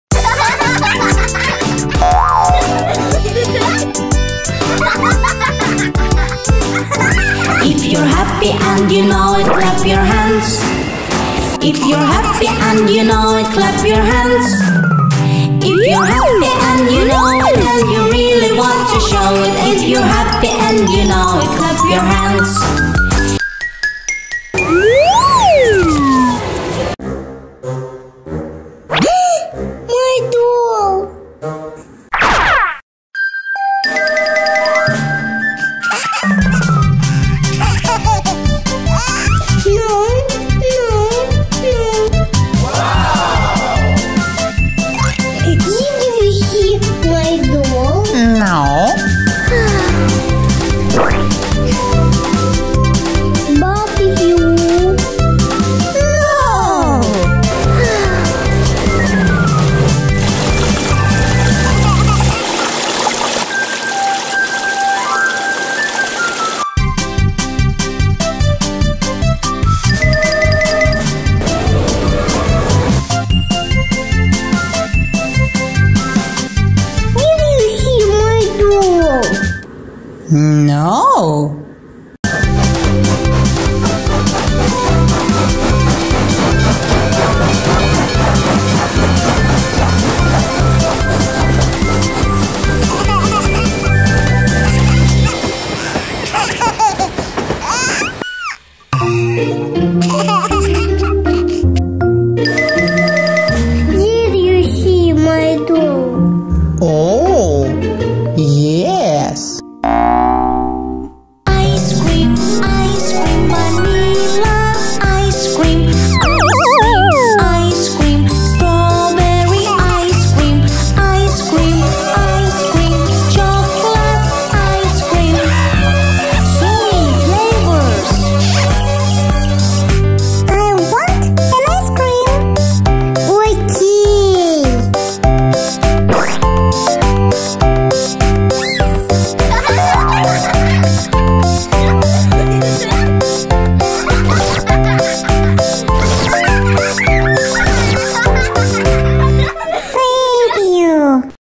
Alena and Icecream shop
Alena plays with a little doll at the playground, but  doll run away and Alena does not know where she is. Alena is going to find little doll, and meet her in the Icecream shop!